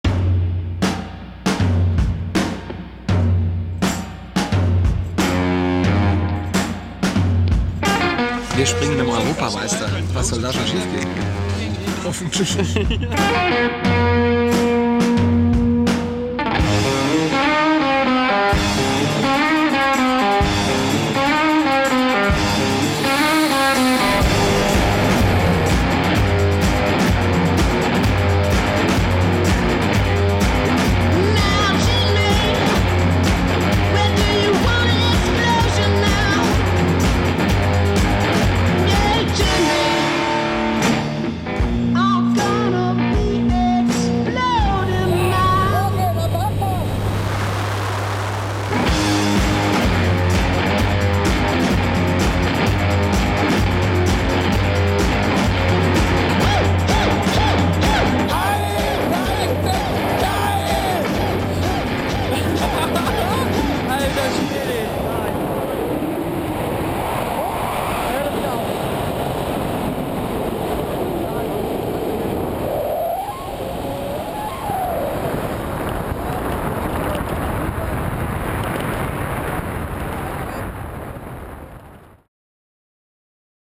mein erstes Mal. Fallschirmspringen mit Europameister Dieter Dankesreiter. YEAH!!!!!
sechzig Sekunden freier Fall aus viertausend fünfhundert Metern Höhe. Das ist einfach nur WOOOOOOOOOOOW! Danke an Dieter Dankesreiter für dieses einmalige Erlebnis. I' ll be back!